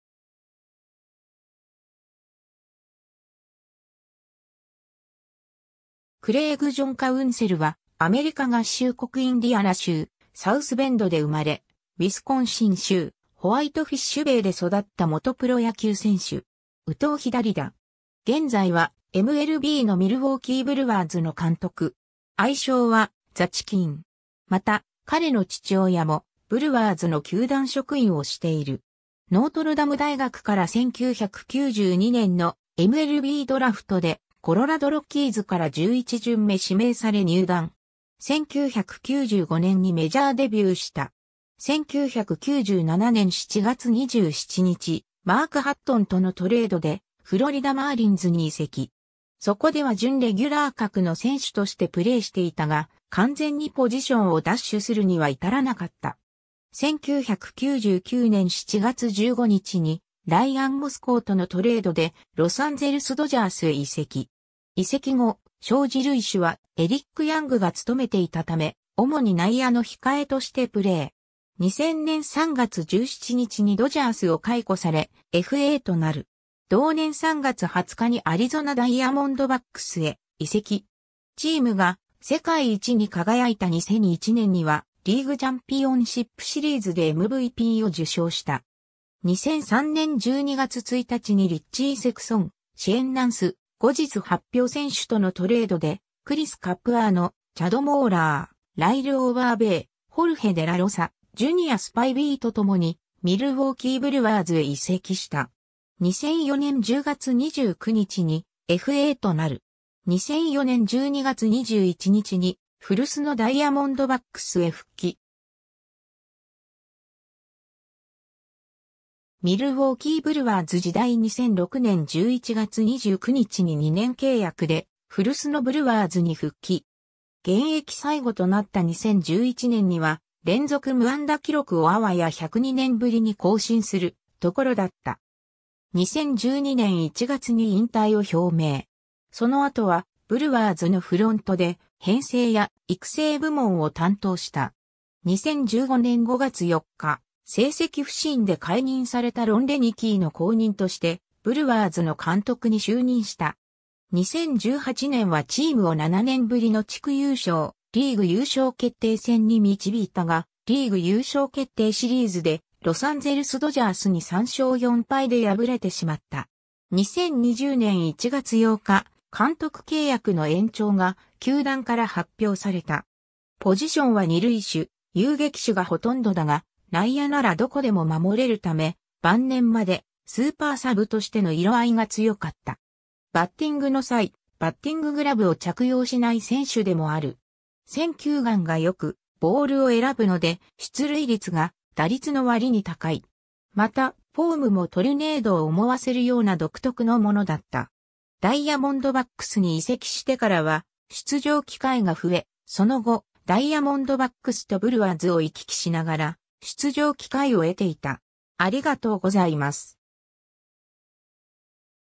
クレイグ・カウンセル
コロラド・ロッキーズ傘下の選手
アリゾナ・ダイヤモンドバックス傘下の選手
クレイグ・ジョン・カウンセル（Craig John Counsell, 1970年8月21日 - ）は、アメリカ合衆国インディアナ州サウスベンドで生まれ、ウィスコンシン州ホワイトフィッシュベイで育った元プロ野球選手（内野手）。
右投左打。
現在は、MLBのミルウォーキー・ブルワーズの監督。
愛称はザ・チキン。
また、彼の父親もブルワーズの球団職員をしている。
 ノートルダム大学から1992年のMLBドラフトでコロラド・ロッキーズから11巡目（全体319位）指名され入団。
1995年にメジャーデビューした。
 1997年7月27日、マーク・ハットンとのトレードでフロリダ・マーリンズに移籍。
そこでは準レギュラー格の選手としてプレーしていたが、完全にポジションを奪取するには至らなかった。
 1999年7月15日にライアン・モスコウとのトレードでロサンゼルス・ドジャースへ移籍。
移籍後、正二塁手はエリック・ヤングが務めていたため、主に内野（二塁手および遊撃手）の控えとしてプレー。
 2000年3月17日にドジャースを解雇され、FAとなる。
同年3月20日にアリゾナ・ダイヤモンドバックスへ移籍。
 チームが世界一に輝いた2001年には、リーグチャンピオンシップシリーズでMVPを受賞した。
 2003年12月1日にリッチー・セクソン、シェーン・ナンス、後日発表選手とのトレードでクリス・カプアーノ、チャド・モーラー、ライル・オーバーベイ、ホルヘ・デラロサ、ジュニア・スパイビーと共にミルウォーキー・ブルワーズへ移籍した。
 2004年10月29日にFAとなる。
 2004年12月21日に古巣のダイヤモンドバックスへ復帰。
 ミルウォーキー・ブルワーズ時代 （2009年5月27日） 2006年11月29日に2年契約で古巣のブルワーズに復帰。
 現役最後となった2011年には、連続無安打記録をあわや102年ぶりに更新するところだった。
 2012年1月に引退を表明。
その後はブルワーズのフロントで編成や育成部門を担当した。
 2015年5月4日、成績不振で解任されたロン・レニキーの後任としてブルワーズの監督に就任した。
 2018年はチームを7年ぶりの地区優勝・リーグ優勝決定戦に導いたがリーグ優勝決定シリーズでロサンゼルス・ドジャースに3勝4敗で敗れてしまった。
 2020年1月8日、監督契約の延長（2023年まで）が球団から発表された。
 ポジションは二塁手、遊撃手が殆どだが、内野ならどこでも守れるため、晩年までスーパーサブとしての色合いが強かった。
 バッティングの際、バッティンググラブを着用しない選手でもある。
選球眼がよく、ボールを選ぶので、出塁率が打率の割に高い。
また、フォームもトルネードを思わせるような独特のものだった。
 ダイヤモンドバックスに移籍してからは出場機会が増え、その後、ダイヤモンドバックスとブルワーズを行き来しながら、出場機会を得ていた。